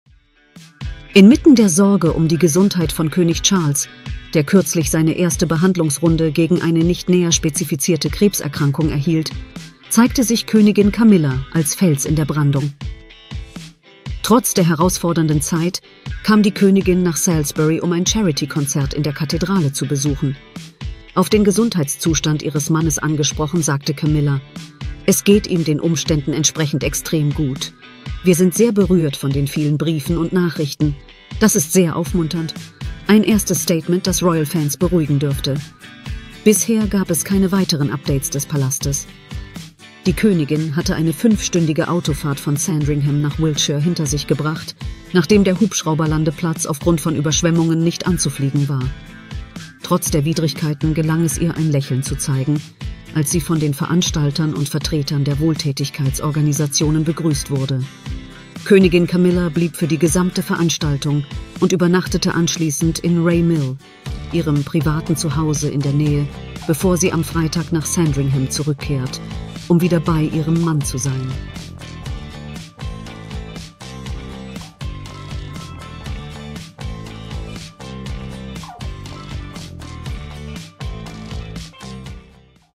Darauf haben wir gewartet! Endlich ein Update zu Charles' Gesundheit • PROMIPOOL
Inmitten der Sorge um die Gesundheit von König Charles, der kürzlich seine erste Behandlungsrunde gegen eine nicht näher spezifizierte Krebserkrankung erhielt, zeigt sich Königin Camilla als Fels in der Brandung. Trotz der herausfordernden Zeit kam die Königin nach Salisbury, um ein Charity-Konzert in der Kathedrale zu besuchen und gab ein Update zu Charles' Gesundheit.